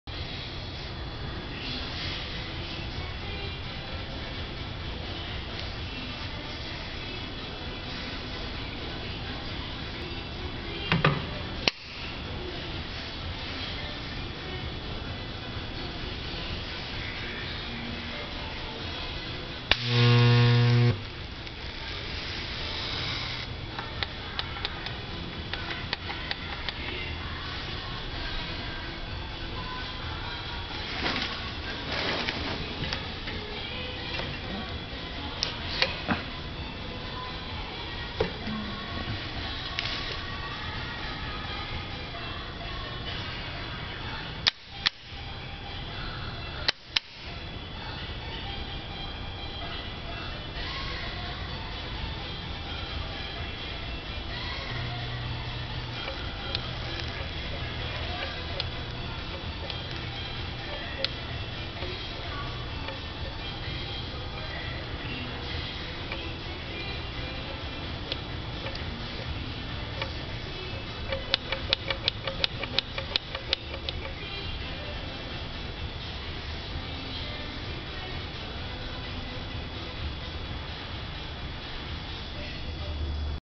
buttweld
New butt welding machine (South Korea)
Purchase Inquiry
Heat treatment welding wire in place without moving from the place running.
The best product in the world (an idea)
Wire 3mm ~ 8mm welding possible
South Korea Samsung welder